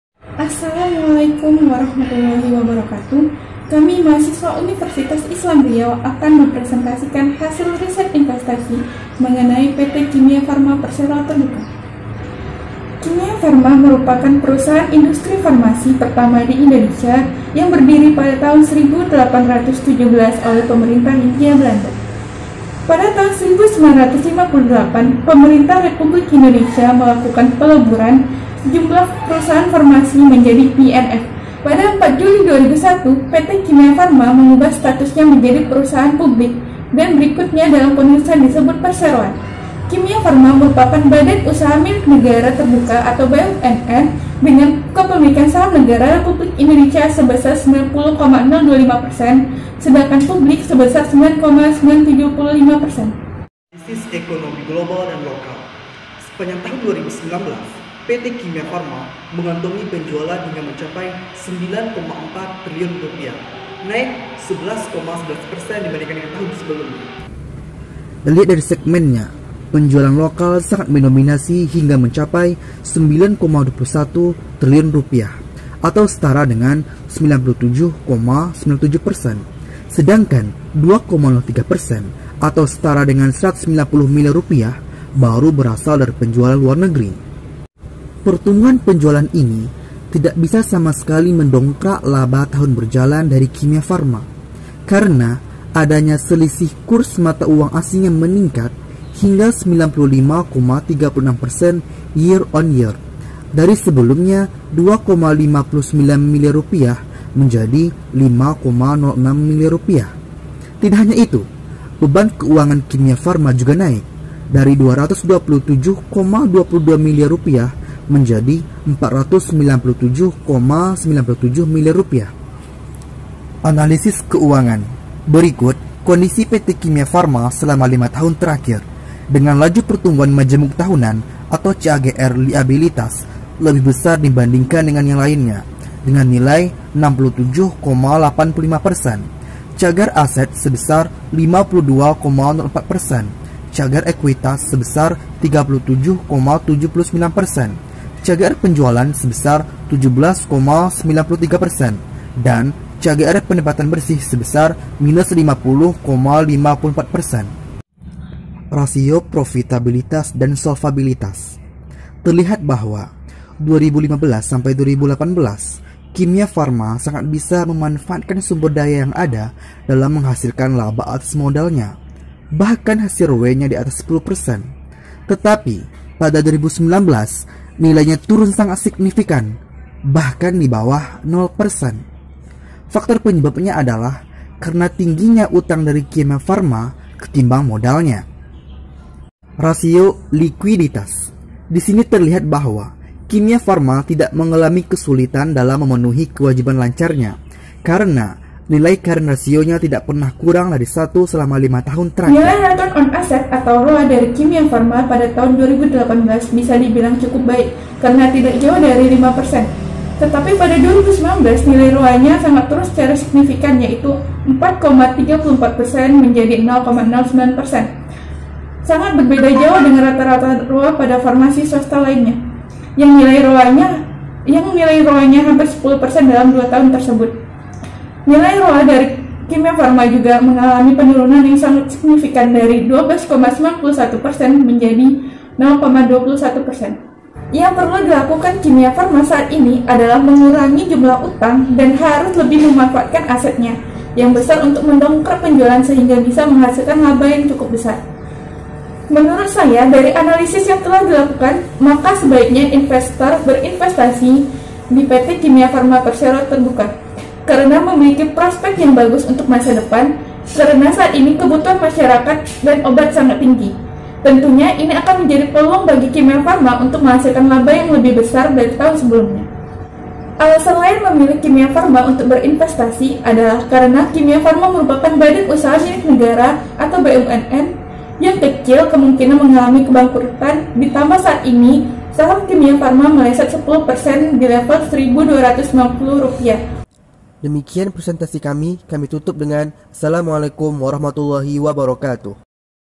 Kimia Farma Industri Farmasi Pemerintah – KBMK 2020
PT Kimia Farma merupakan industri farmasi pertama di Indonesia yang memiliki prospek yg bagus bagi investor dalam berinvestasi karena memiliki aset yang cukup besar ditambah badan usaha milik negara(BUMN) yg kecil kemungkinan mengalami kebangkurat